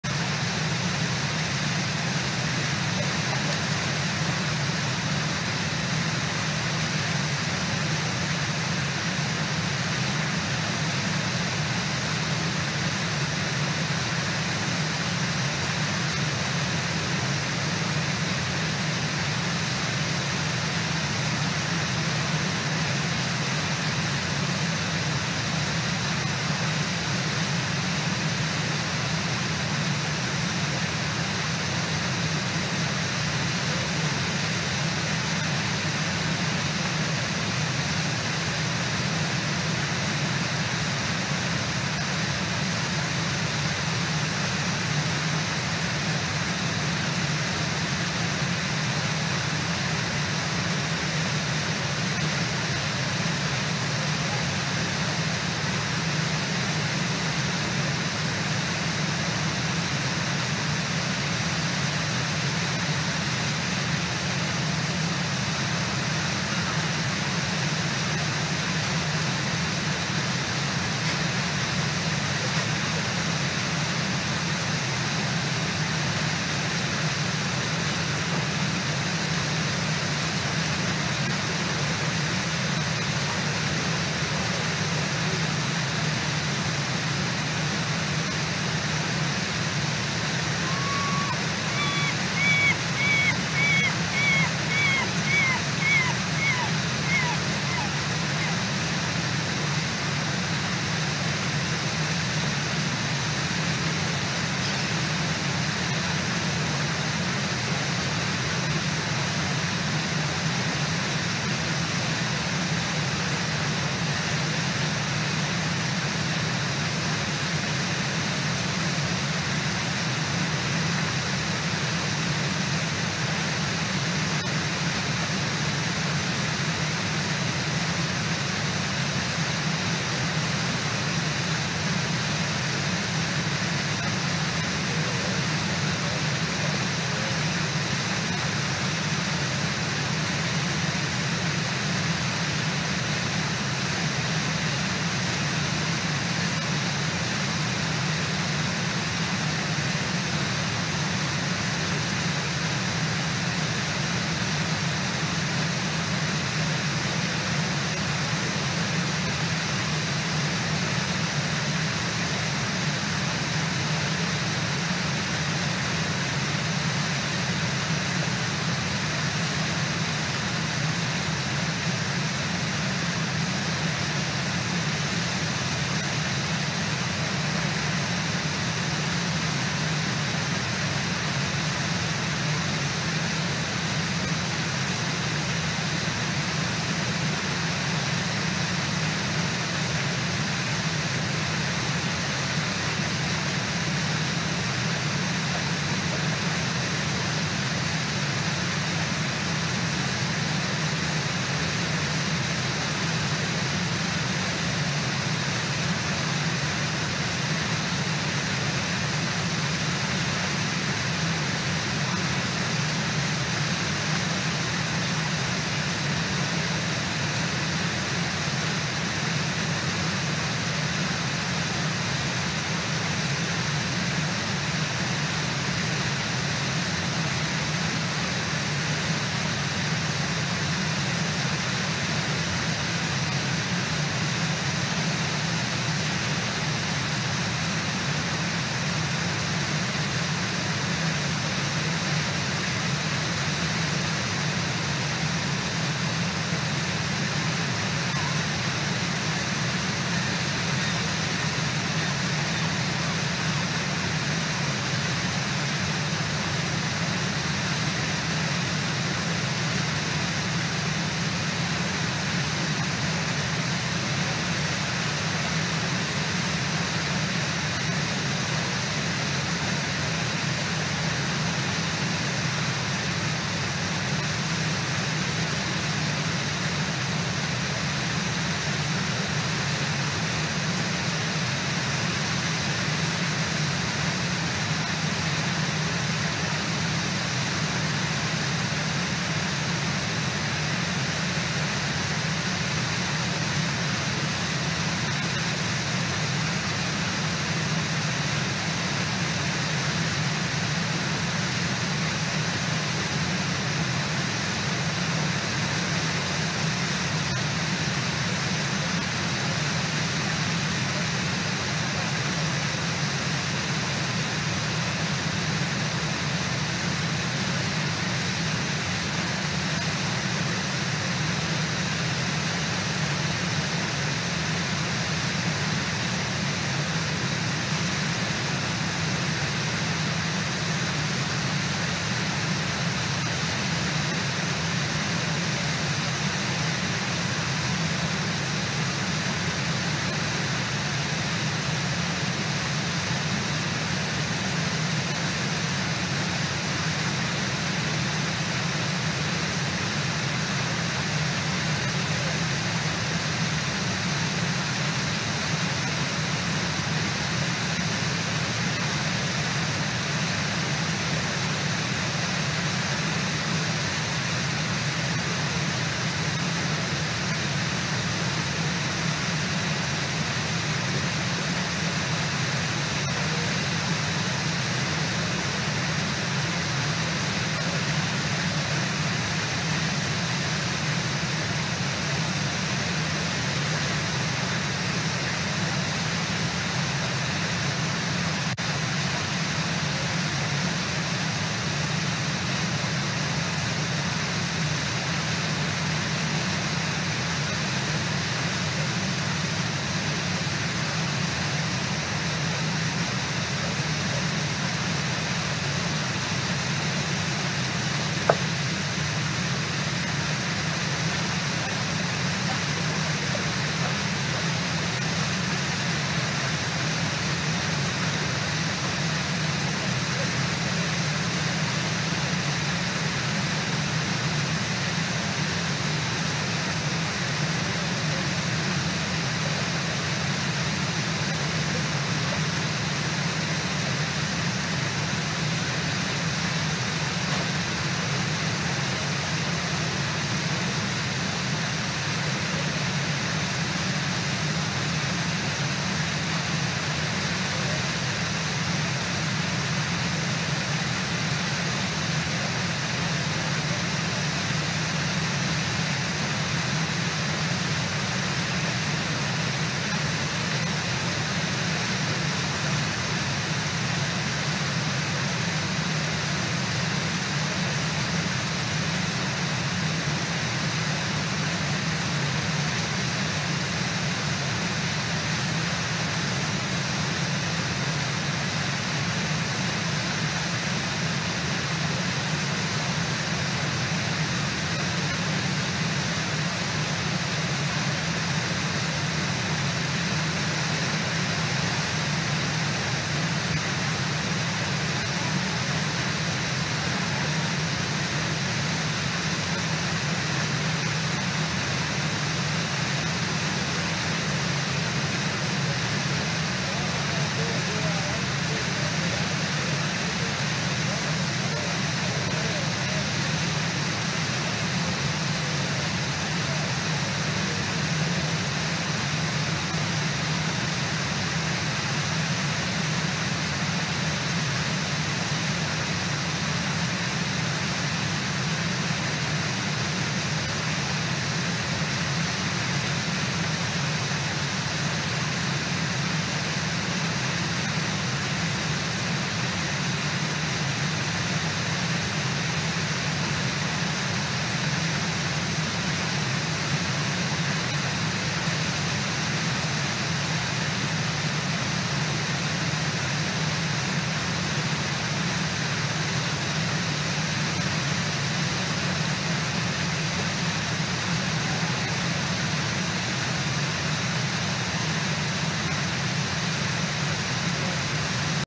8/12/2022 BFL 910 & Lil Bead Explore.org
Watch salmon leaping up the falls, while brown bears compete with each other for the best fishing spots. The largest and most successful bears can catch and eat more than 30 salmon (over 120 pounds) per day! Bears are most abundant at Brooks Falls in late June and July during the sockeye salmon migration, but also keep an eye out for bald eagles, lots of gulls, and maybe even the occasional wolf trying to partake in the salmon buffet.